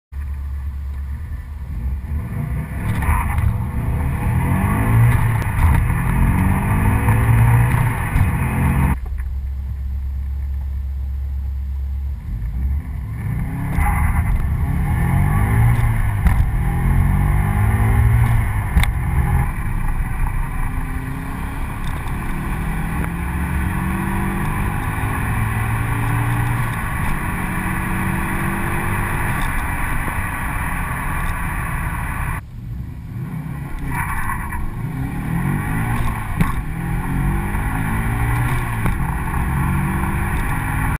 Subaru Impreza WRX STI Type-R launch 0-100 km/h
1998 Subaru Impreza WRX STI Type R Version 4 launch from 0-100 km/h. Launched at about 3-4k rpm. The car has a 3" exhaust, GEMS ecu, blowoff. Running on winter tires 195/65/15 with the DCCD set to 35/65. With sloppy gear ganges and not using antilang the car manages 0-100 in under 5 seconds. Filmed with GoPro HD Hero.